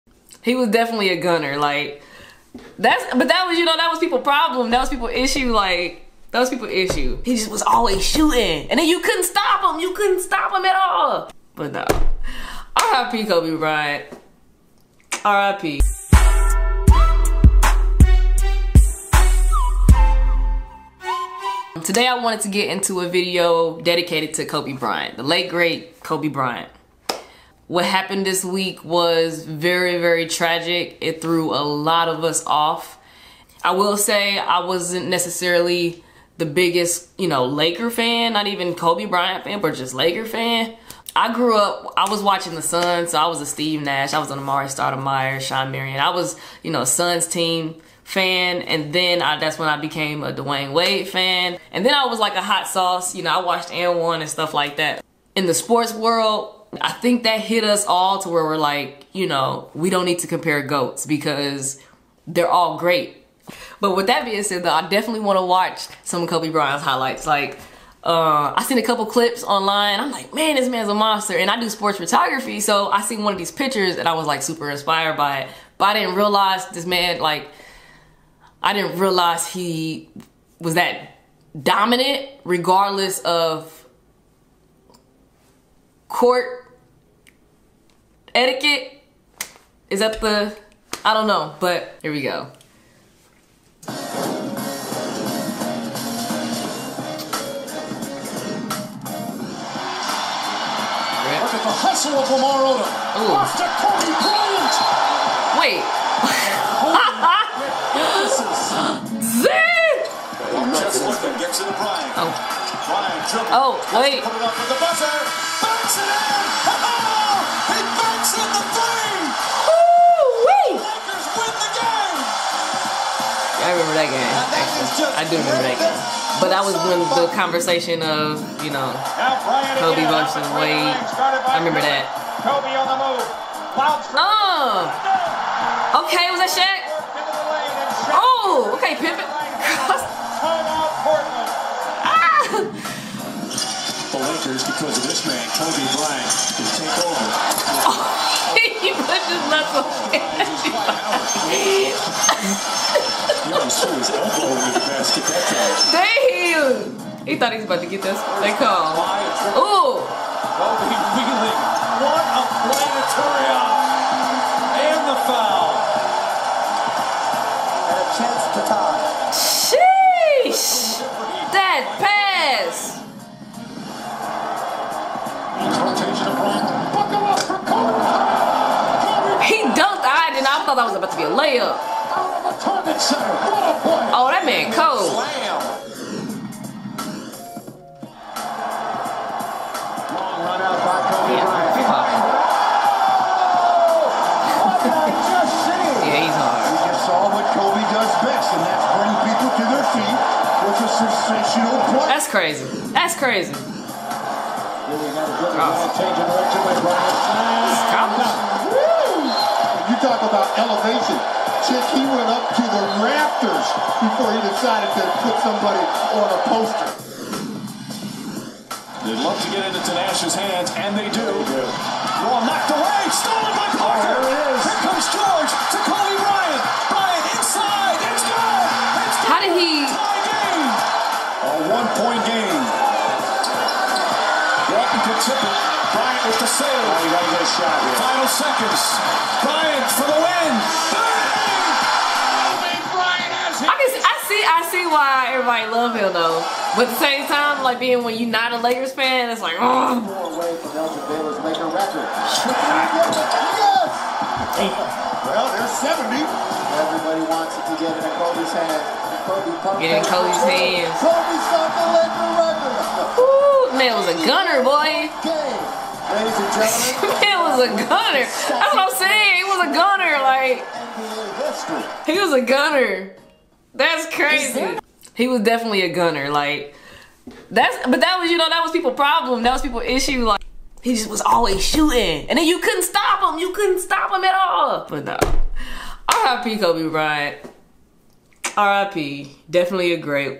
Kobe Bryant Highlights [REACTION]
RIP KOBE

I decided to watch some of Kobe Bryant's (RIP) highlights to honor his legacy in the sports world.

As I want to continue building within discussing sports topics, I didn't want to skip over the opportunity to at least study his game and pay my respects.

To all Kobe Bryant fans, I am truly sorry for your loss.
RIP to all of the 9 victims involved. That really hurt my heart.